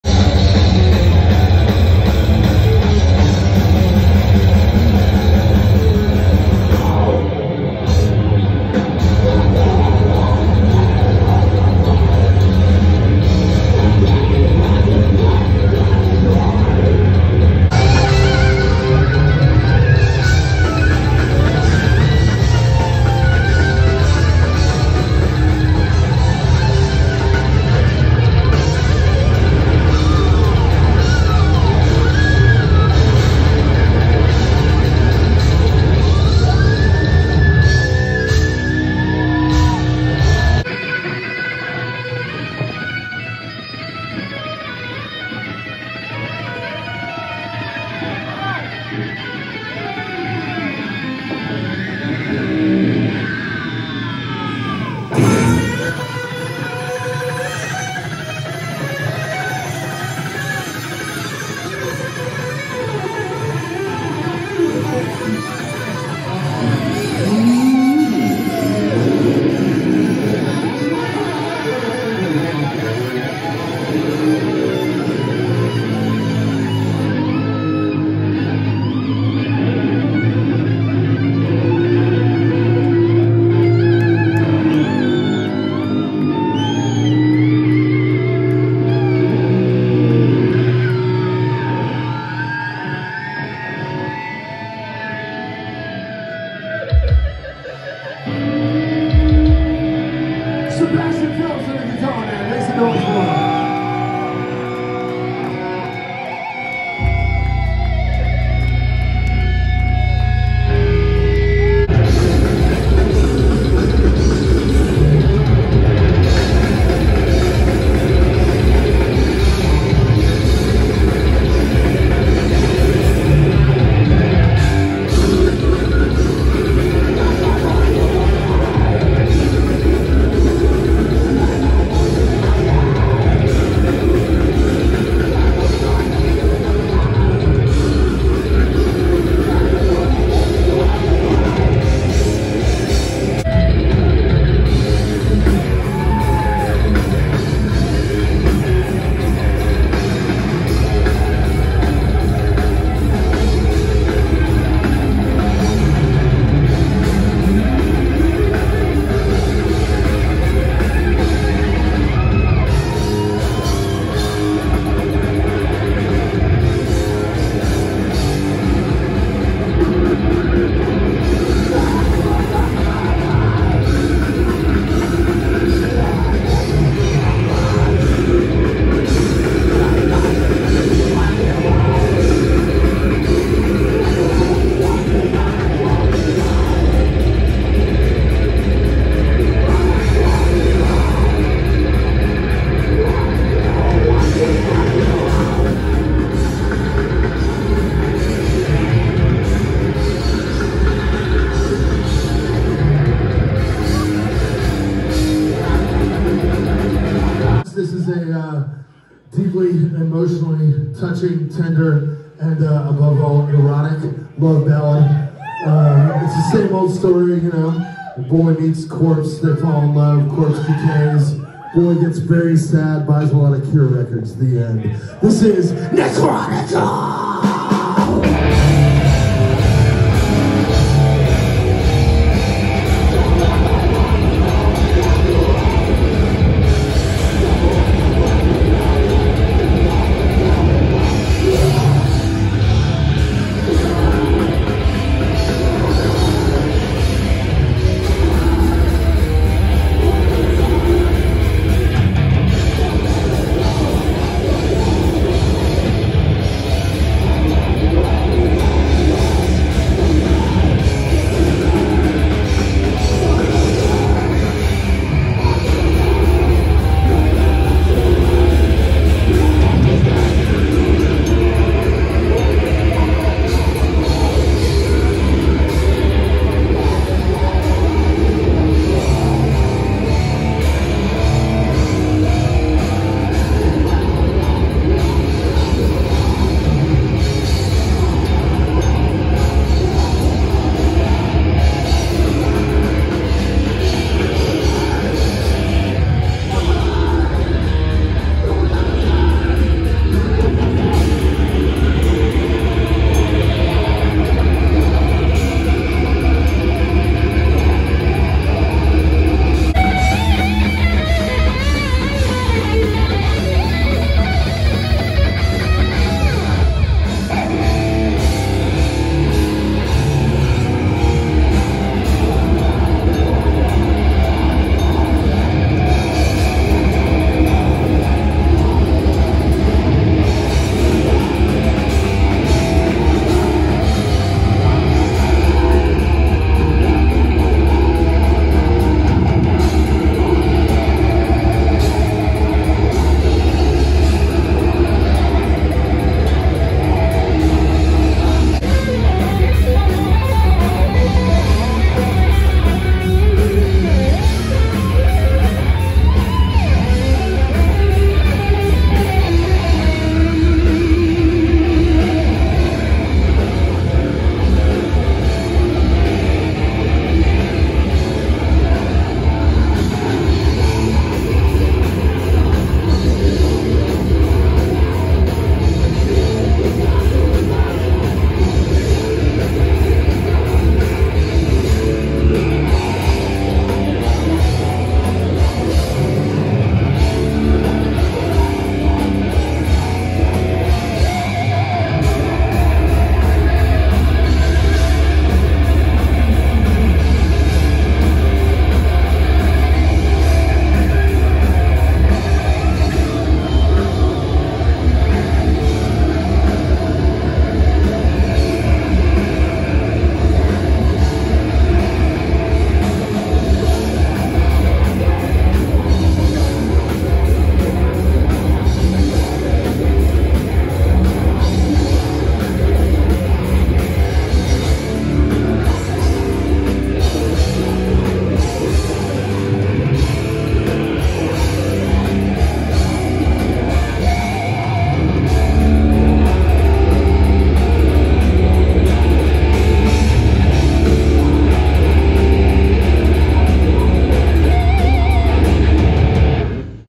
Exhumed live in Austin, TX 2022
Exhumed performing live in Austin, Texas at Come And Take It Live (12/8/2022)